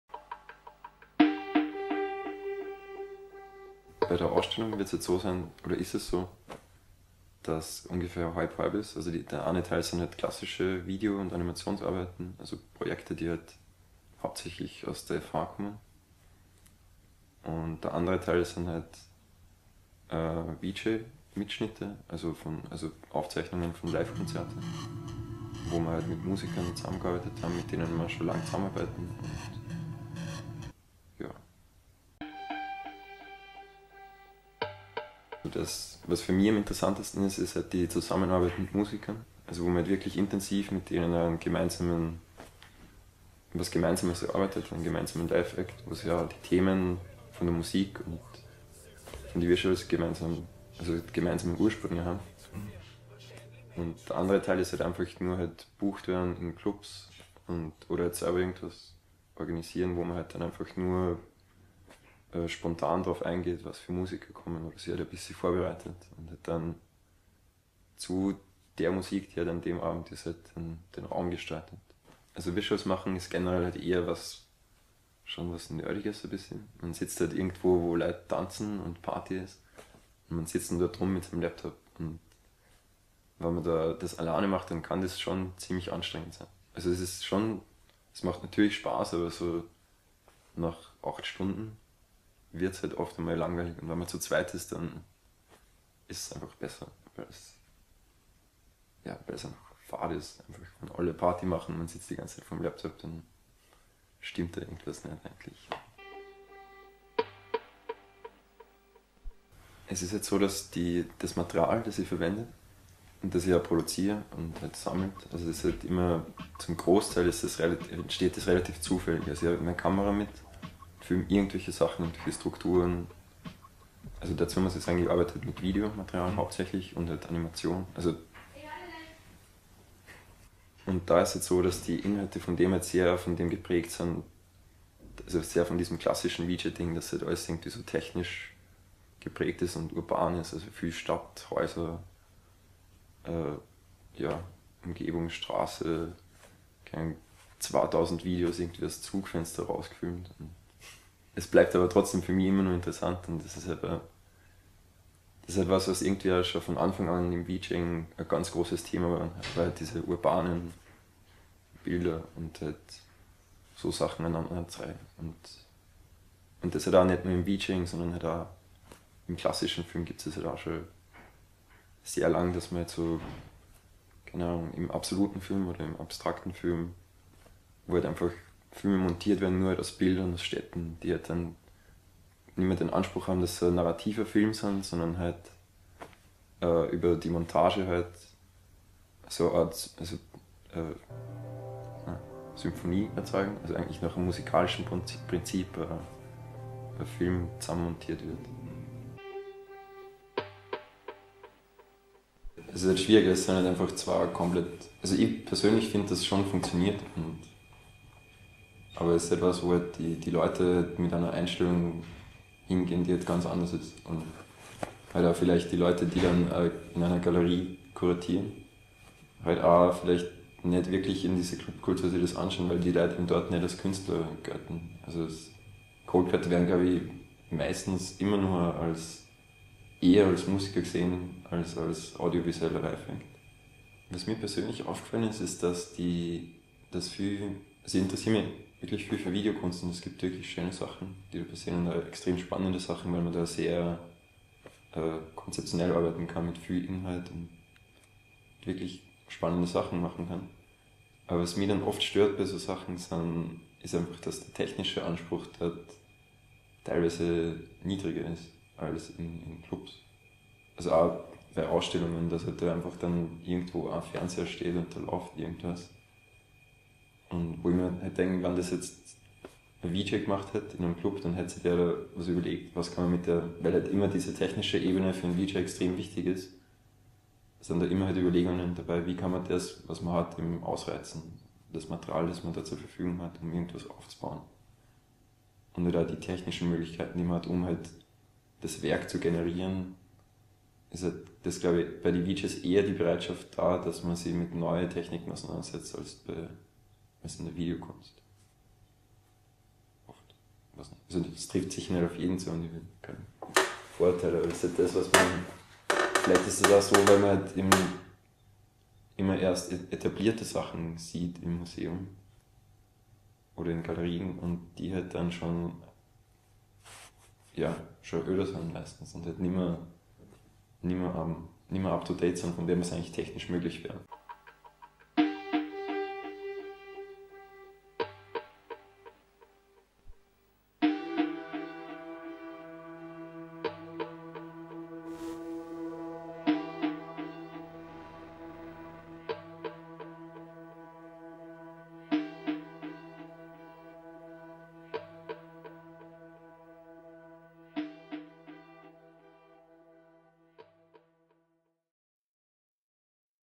Local Heroes 2009 - Interview Martin Pühringer
Local Heroes 2009 -
Interview Martin Pühringer
Galerie der Stadt Wels 2009
Medien Kultur Haus Wels